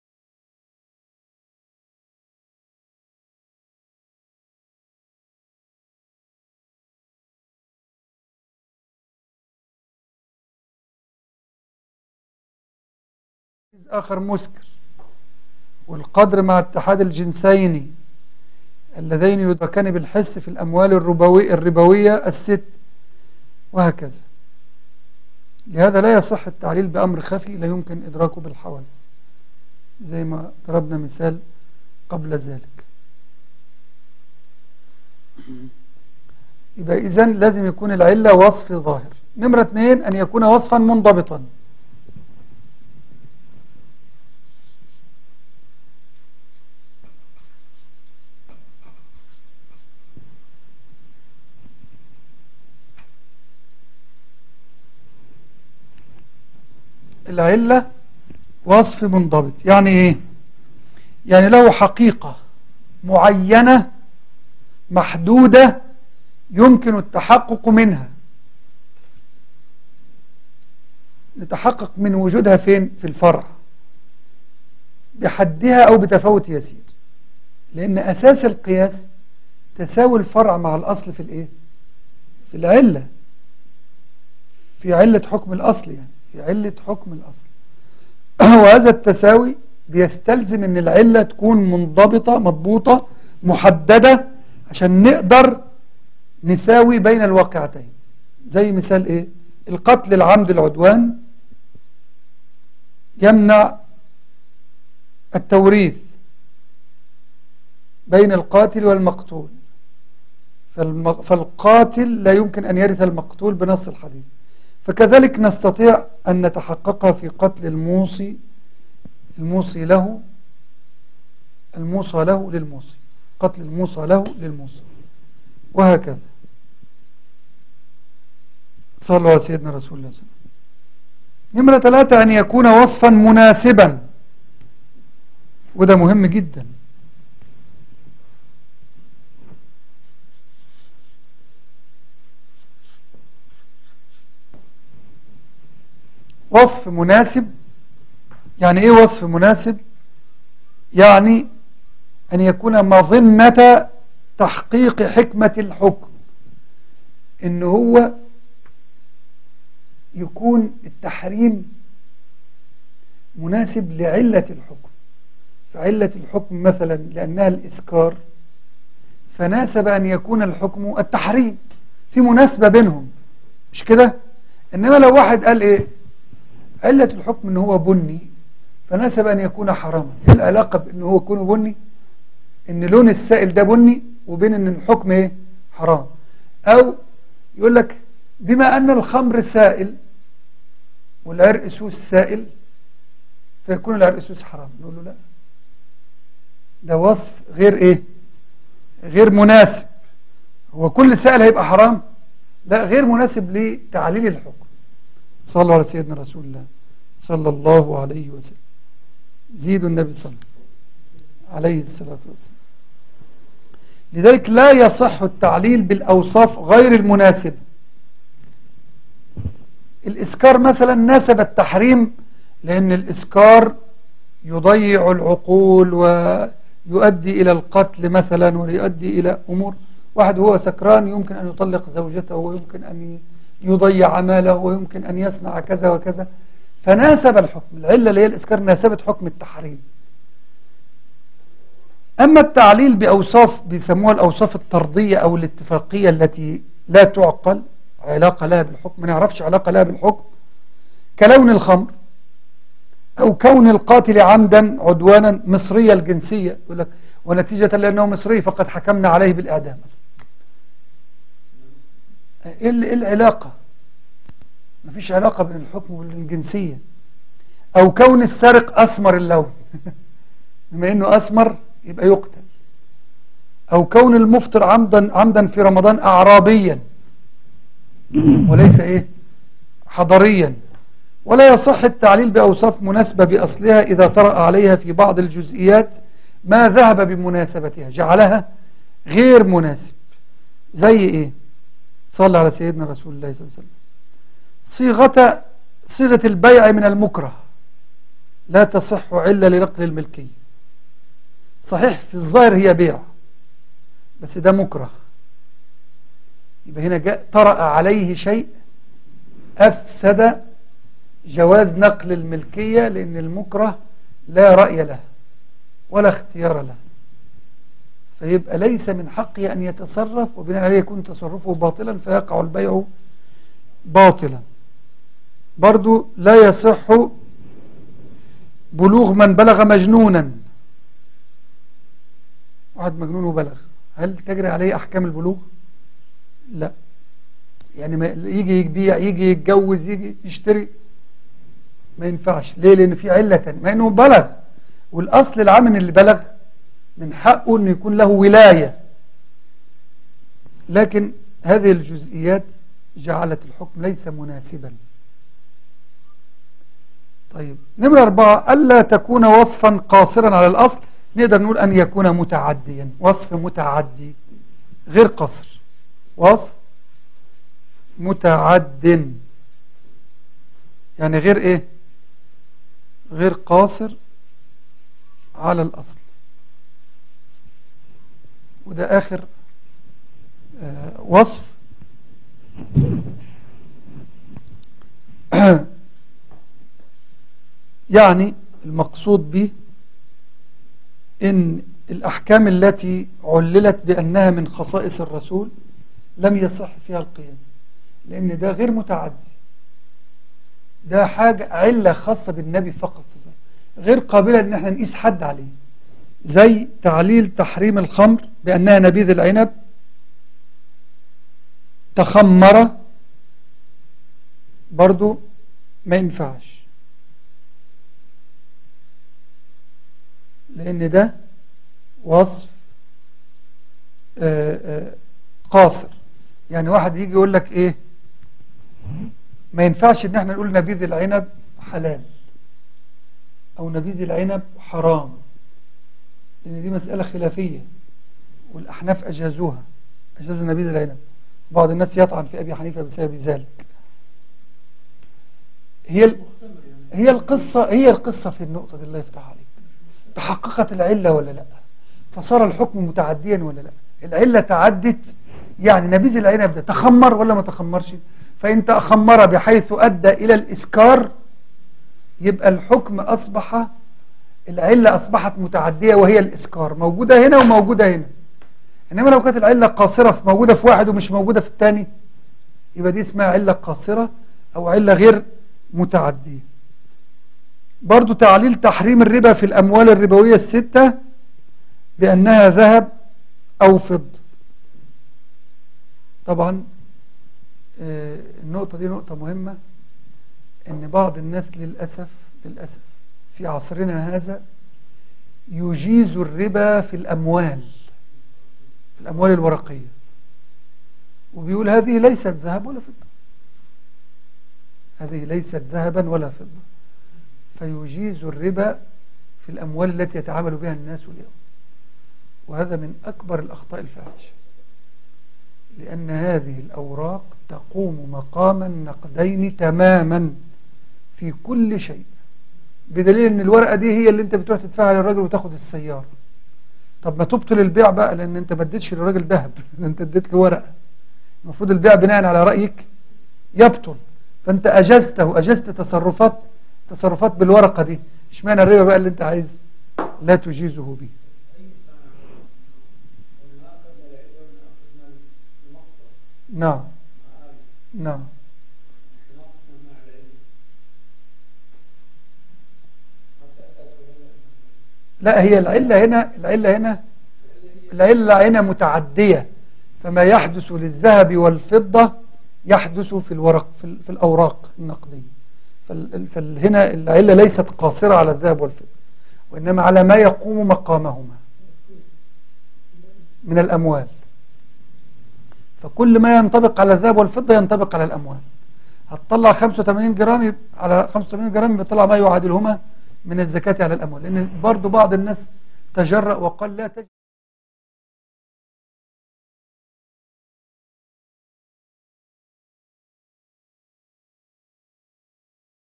الدورة الشرعية في علم أصول الفقه - اليوم الخامس - ج 7
تم عقد هذه الدورة الشرعية في علم أصول الفقه في مسجد الجمعية الإسلامية جيسن بمدينة جيسن الألمانية في عام 2009
لمزيد من المعلومات عن الجمعية إضغط الرابط التالي

ولمزيد من المعلومات عن الدكتور منير جمعة إضغط هذا الرابط التالي